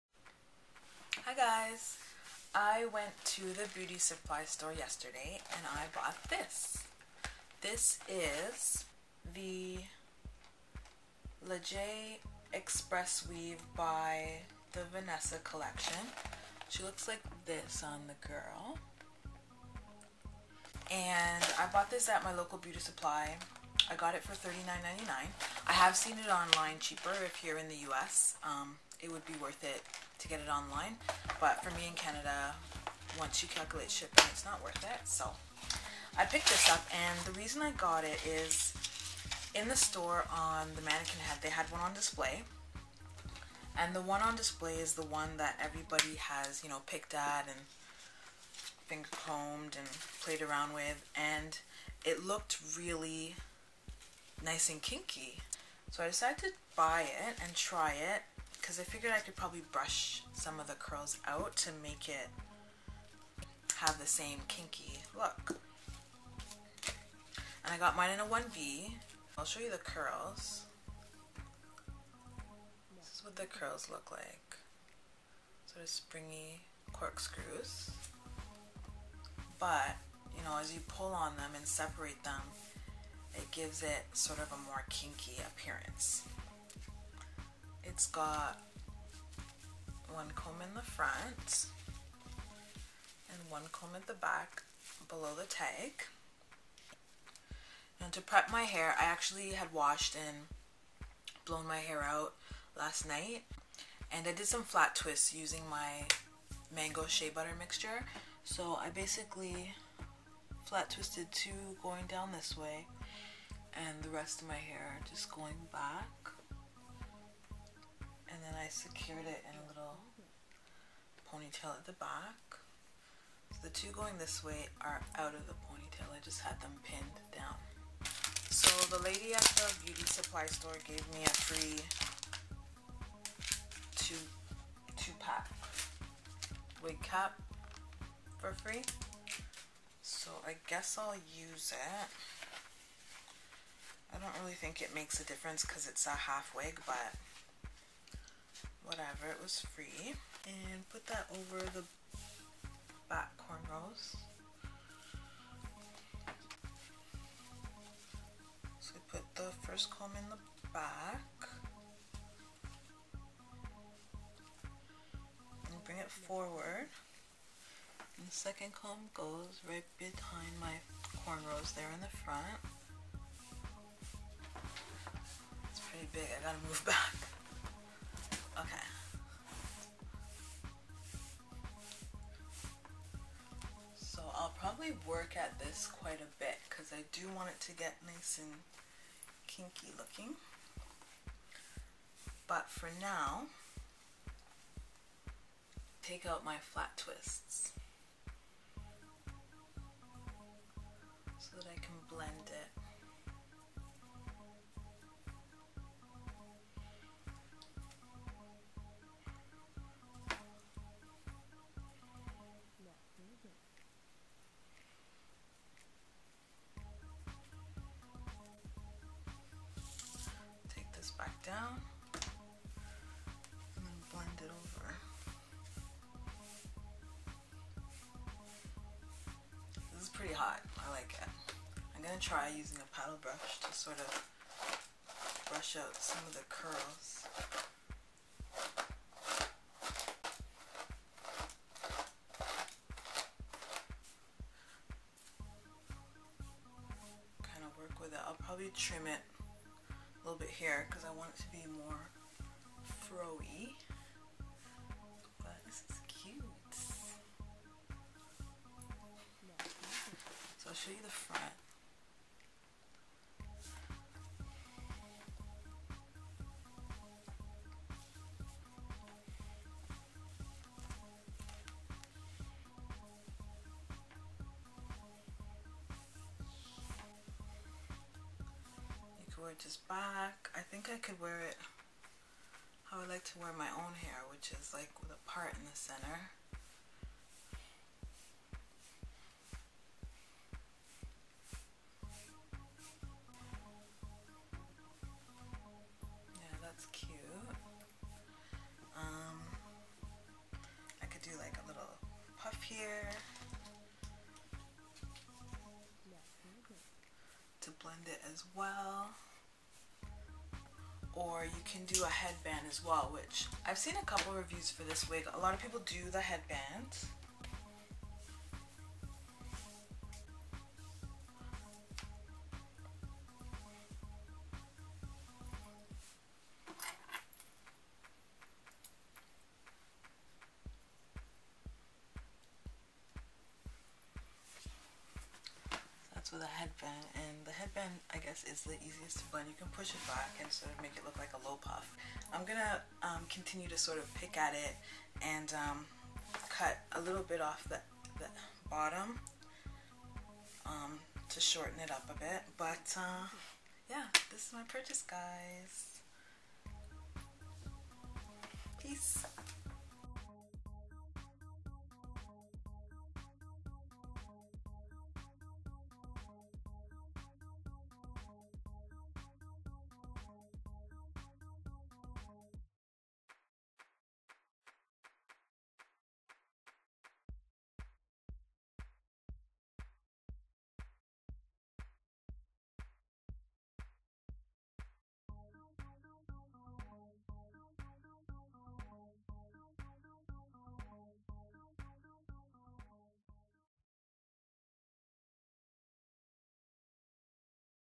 LA JAY HALF WIG REVIEW -BY VANESSA COLLECTION
HALF WIG REVIEW
Style : Lajay
Brand: Vanessa Collection
Colour: 1B
Cost: $39